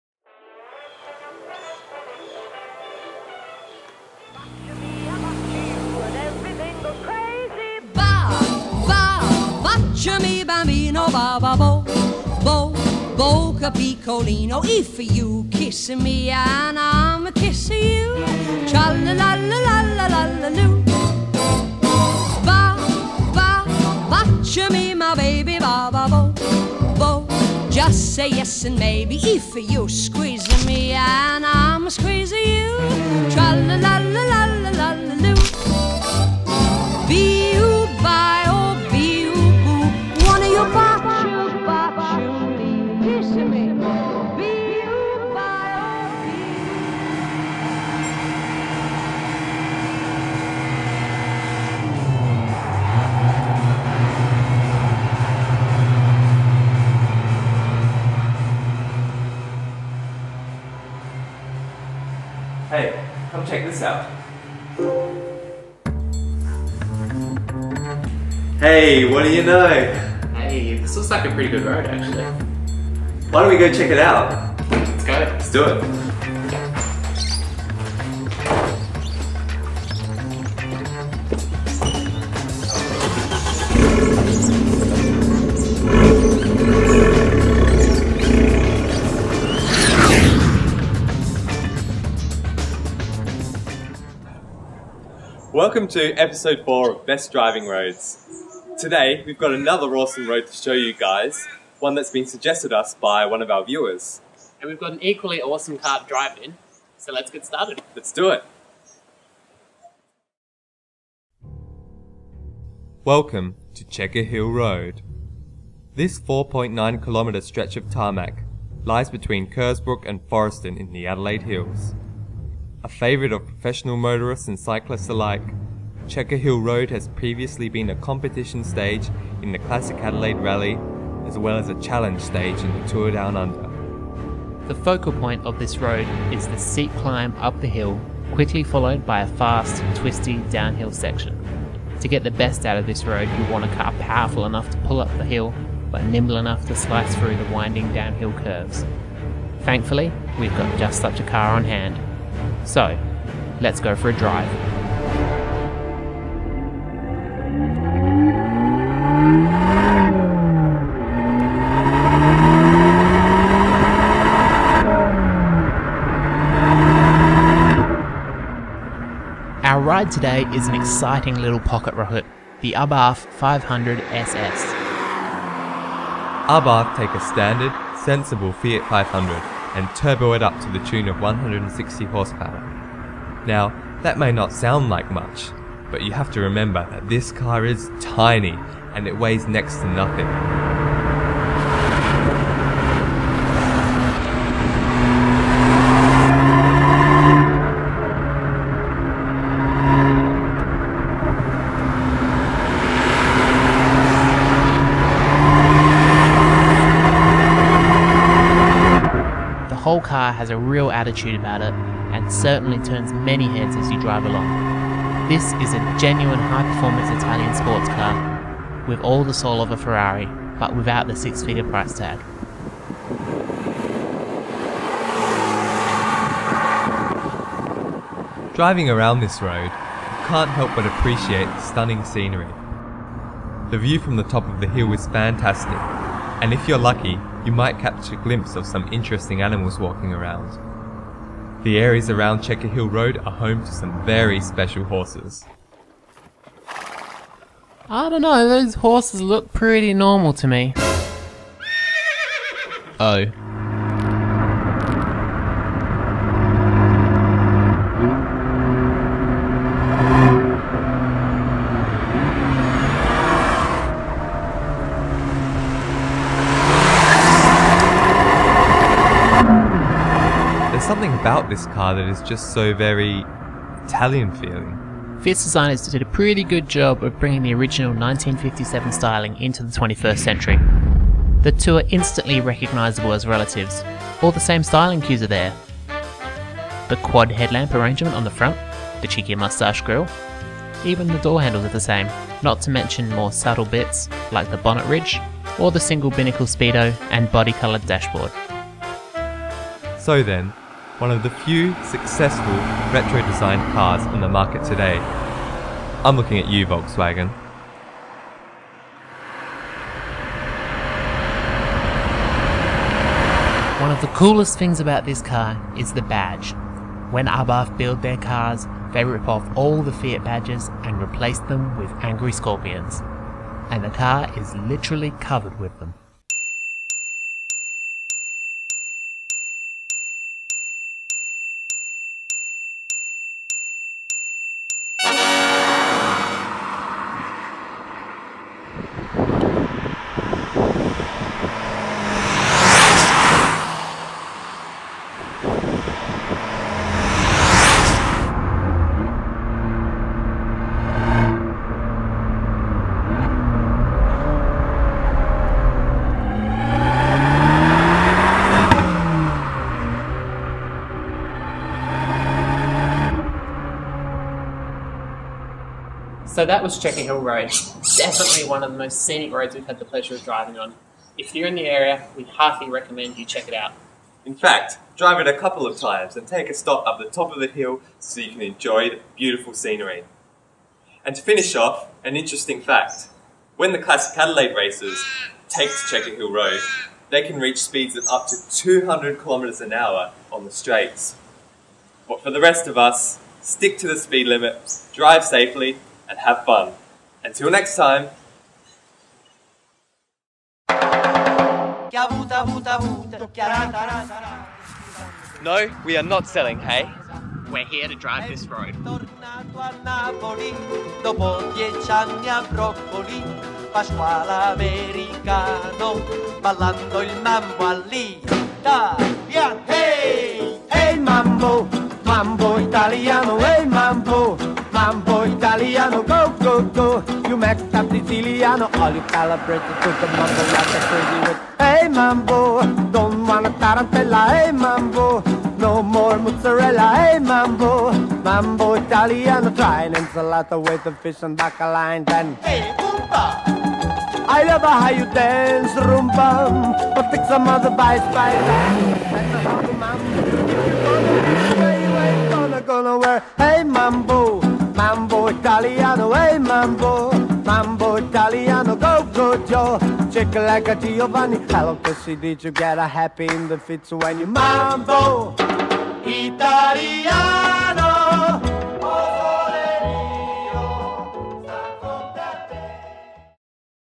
Fiat 500 Abarth on Checker Hill Road - Best Driving Roads 04
In our long-awaited fourth episode, we take a Fiat 500 Abarth Esseesse to a great hills road with a mad uphill dash followed by a twisty downhill section, Checker Hill Road in the Adelaide Hills.

Like our new page on Facebook to stay up to date with Best Driving Roads!

Those interested in checking out Checker Hill Road for themselves should go